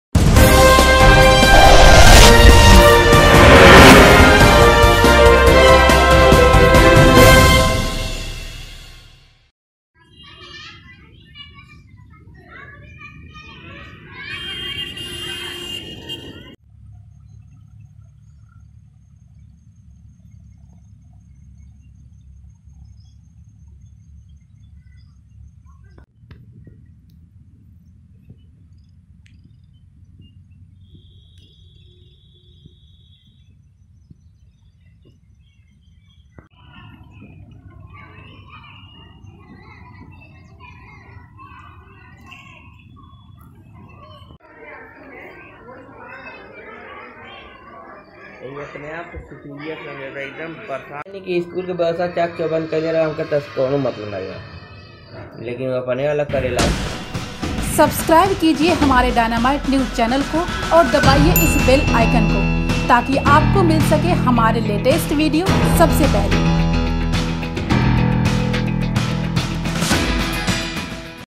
महराजगंज: प्राथमिक विद्यालय में गंदगी का लगा अंबार
महराजगंज: सिसवा विकासखंड क्षेत्र ग्राम सभा बरवा द्वारिका के प्राथमिक विद्यालय में गंदगी का अंबार लगा हुआ है। विद्यालय के प्रधानाचार्य इसकी शिकायत उच्चाधिकारियों से कर चुके हैं। लेकिन कोई सुनवाई नहीं हो रही।